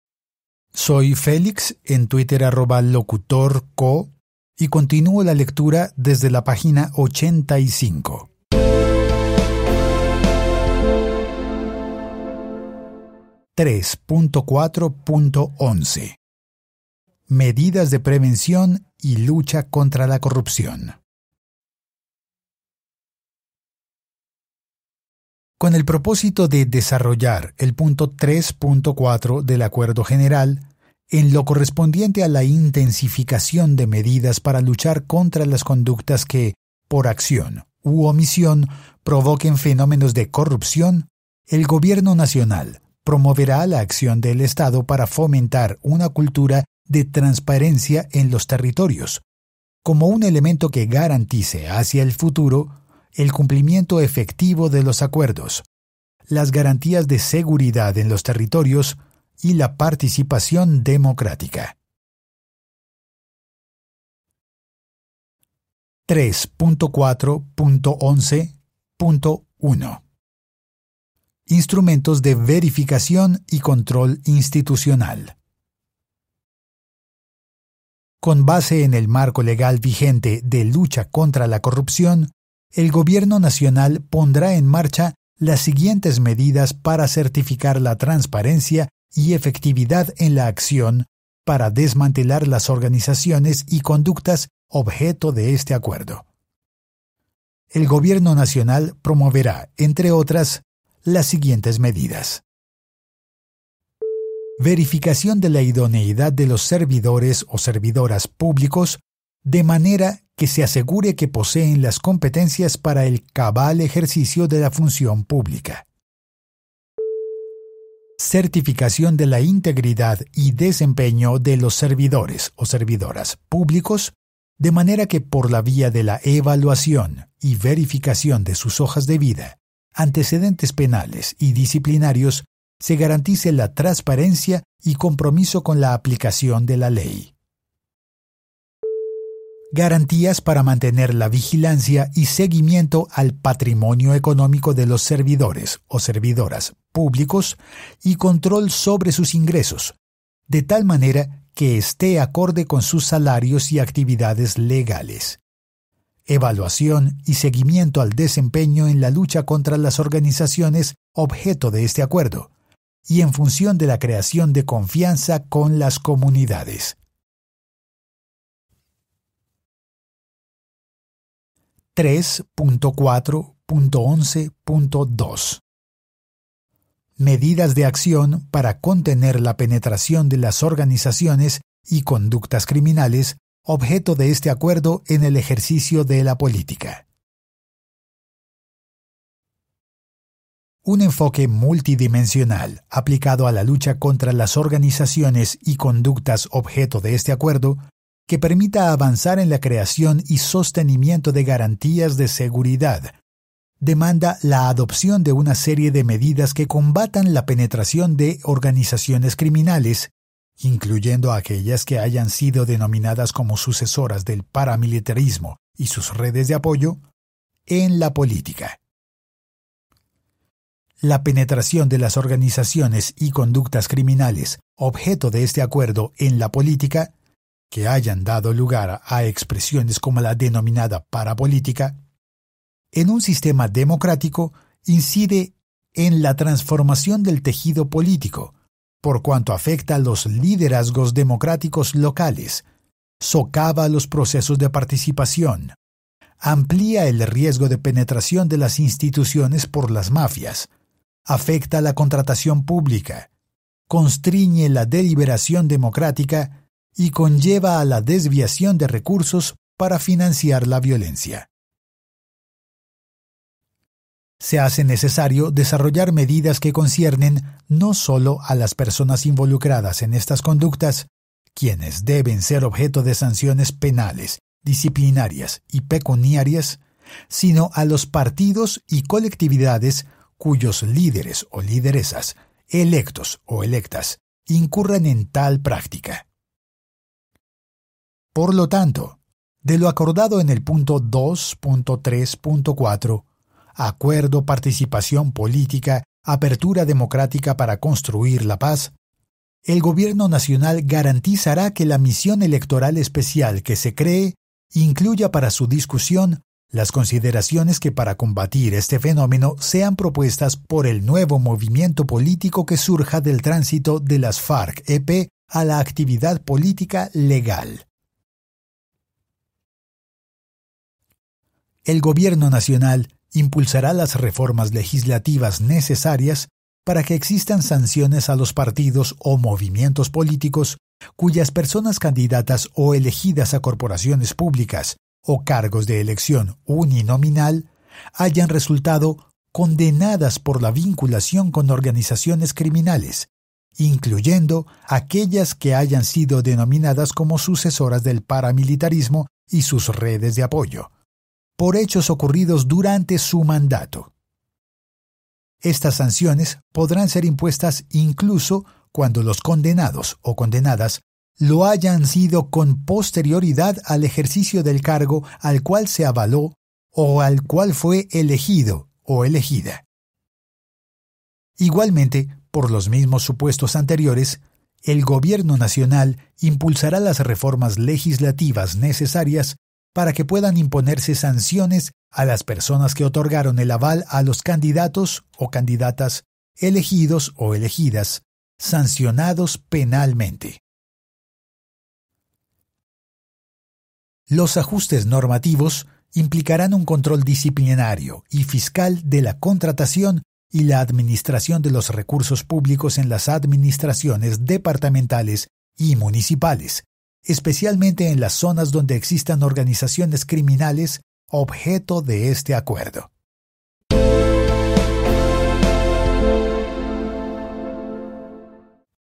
3.4.11 Medidas de prevención y lucha contra la corrupción
Leído por @LocutorCo

3.4.11 Medidas de prevención y lucha contra la corrupción
3.4.11.1 Instrumentos de verificación y control institucional
3.4.11.2 Medidas de acción para contener la penetración de las organizaciones y conductas criminales objeto de este acuerdo en el ejercicio de la política

Lectura del ACUERDO FINAL PARA LA TERMINACIÓN DEL CONFLICTO Y LA CONSTRUCCIÓN DE UNA PAZ ESTABLE Y DURADERA